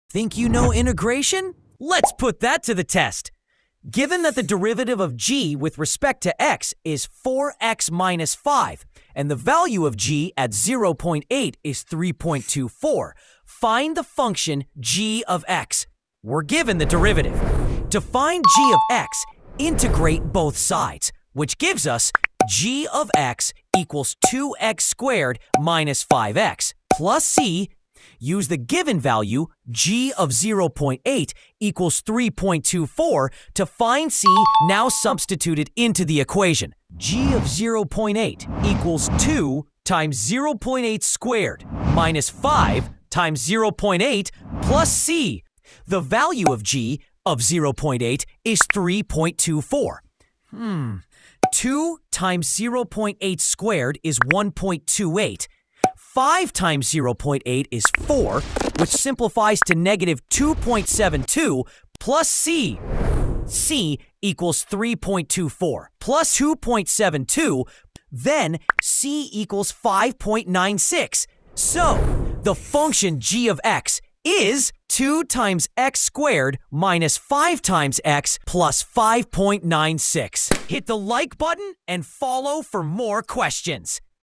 Can You Solve This Integration Problem? | Find g(x) Given g'(x) and a Point
Think you’ve mastered integration? Let’s test your calculus skills with this powerful problem on finding a function from its derivative!

In this video, we’re given:
📌 g′(x) = 4x - 5
📌 g(0.8) = 3.24

Watch step-by-step as we:
✅ Integrate g′(x) to find g(x)
✅ Use the given point to solve for the constant of integration
✅ Arrive at the final function: g(x) = 2x² - 5x + 5.96

📚 Concepts Covered:

Basic integration techniques

Initial conditions and constants of integration

Substitution and simplification in calculus

✨ Perfect for:

High school and college students studying calculus

Anyone preparing for competitive exams (like SAT, JEE, AP Calculus)

Math enthusiasts looking to sharpen their integration skills

💬 Drop a comment if you solved it before the explanation
📤 Share with a friend who loves math challenges